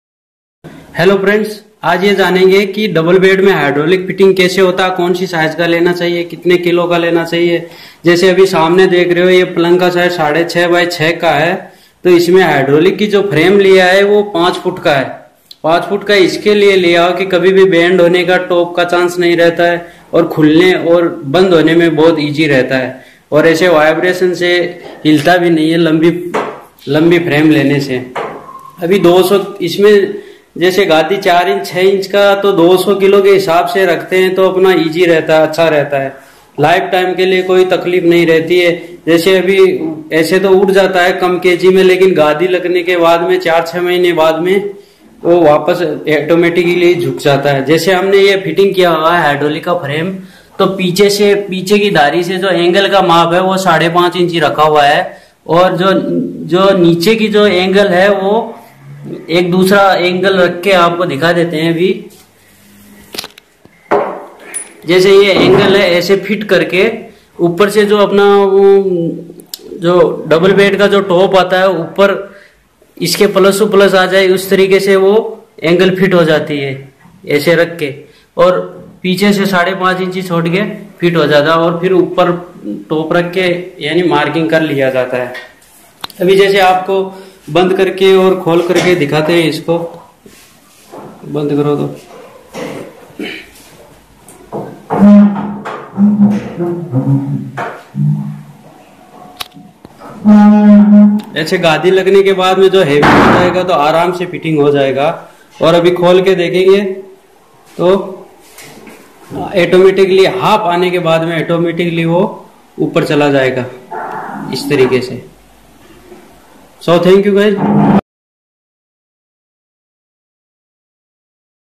Hydraulic bed fitting design and cost ! Dubel bed me hydraulic pump lagane ka tarika
This video about for hydraulic pump fitting and desine
Hydraulic fitting idea
Dubel bed design
Bed size 5.5'×6 feet